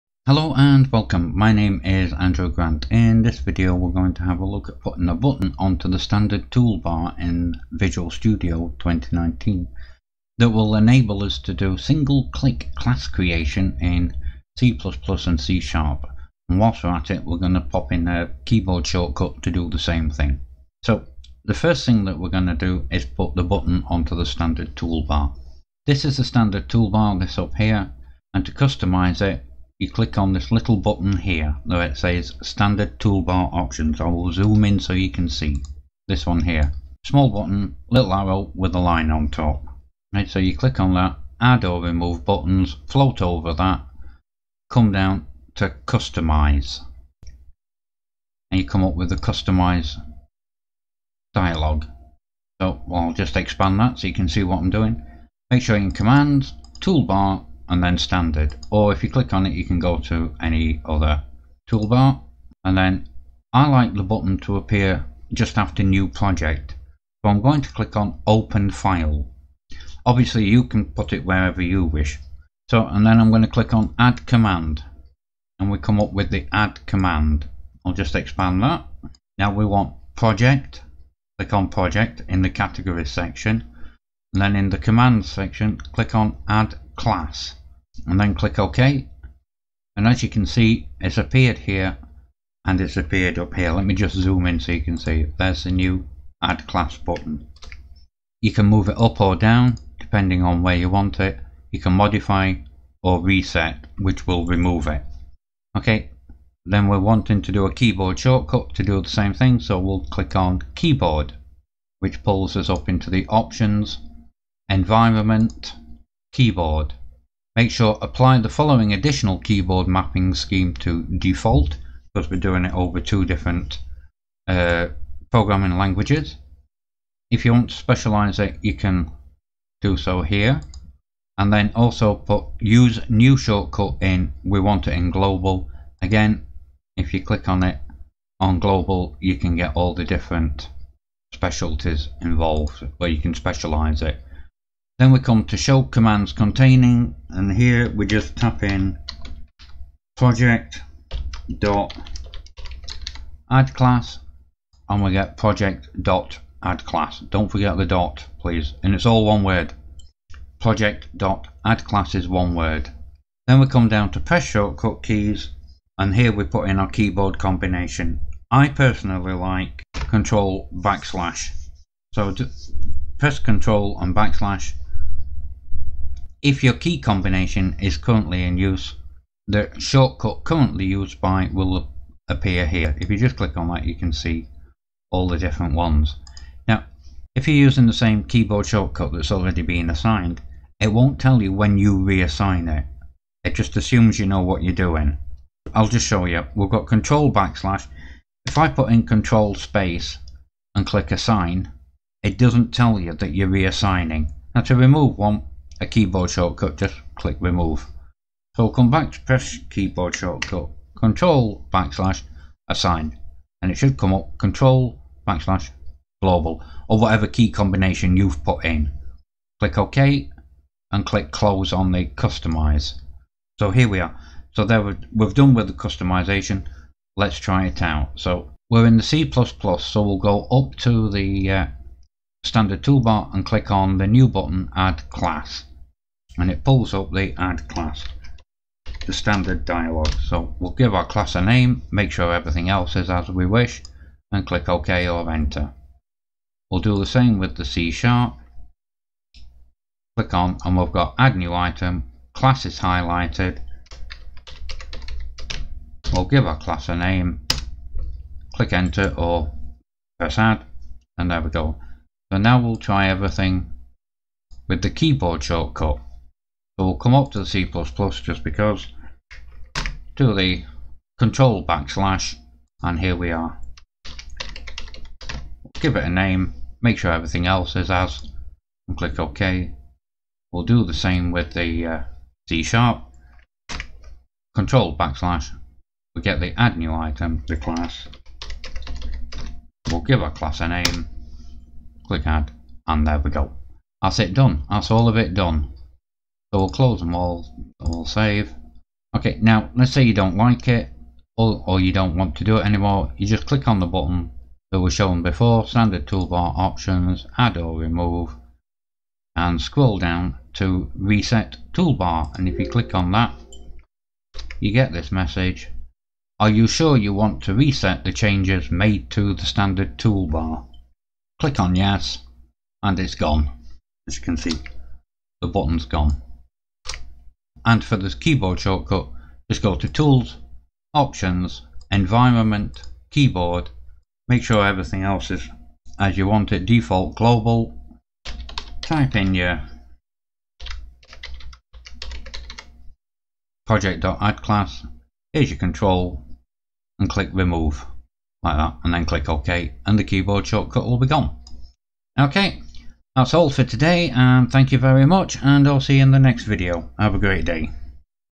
Single Click and Keyboard Combination Shortcut, Class Creation in Visual Studio 2019
Create a button, and assign a keyboard combination shortcut,  to do single click class creation in Visual Studio 2019 for C++ and C# programming.